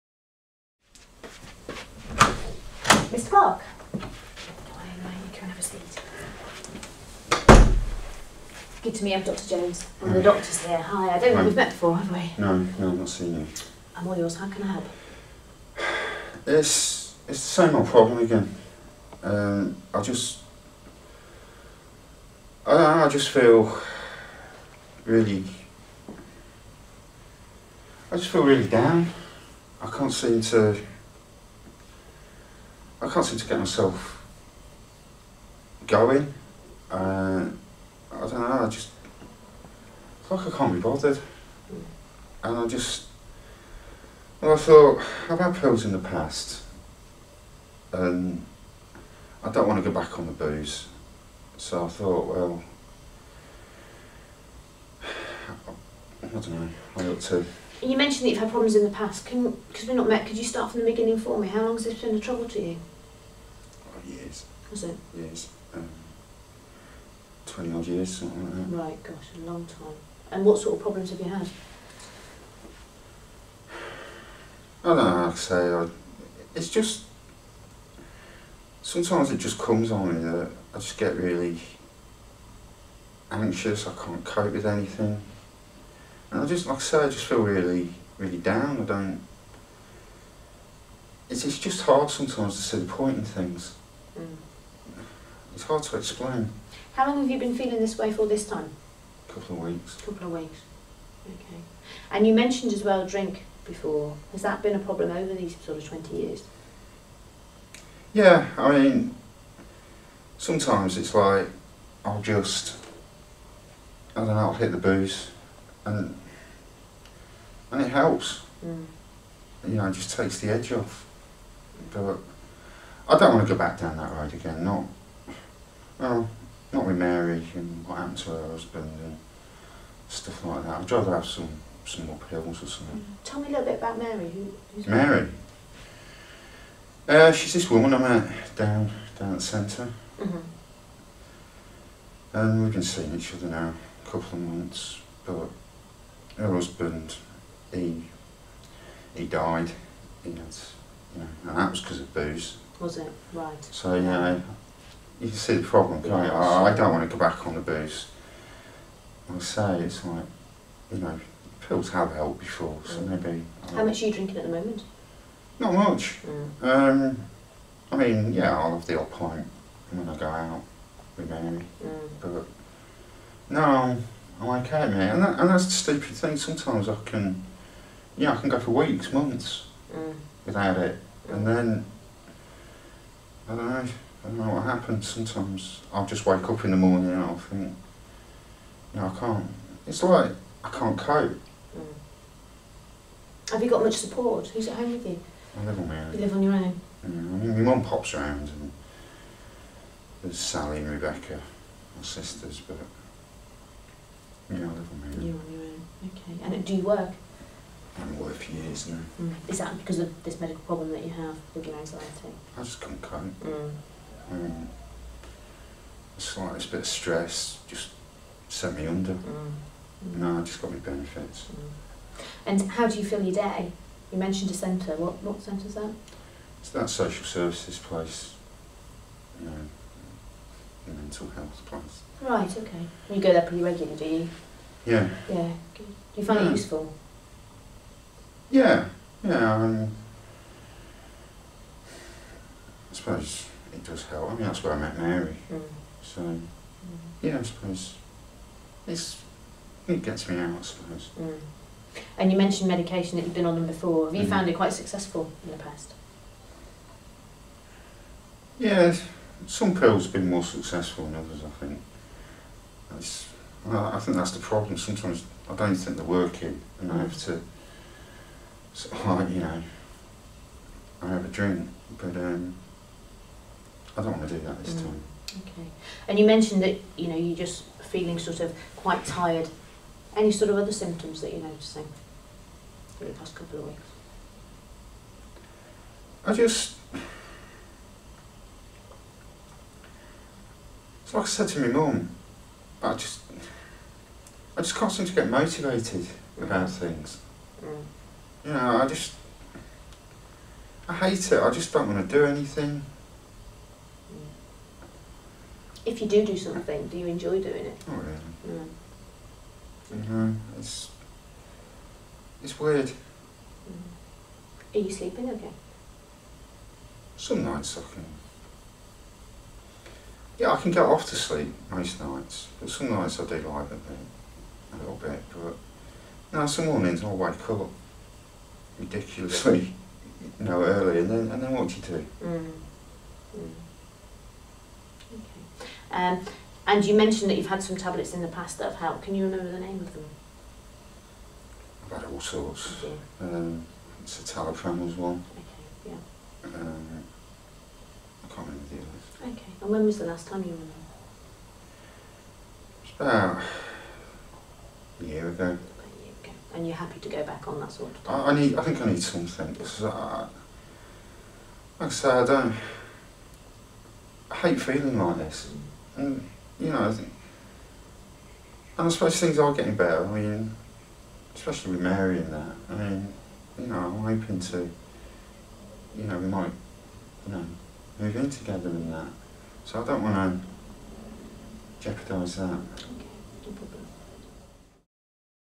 Alcohol and depression: Consultation 1
This series of films provides the opportunity for GPs in training and post-qualifying social workers in mental health to share perspectives on appropriate interventions with service users presenting with depression in primary care. Filmed by the Centre of Excellence in Interdisciplinary Mental Health (CEIMH).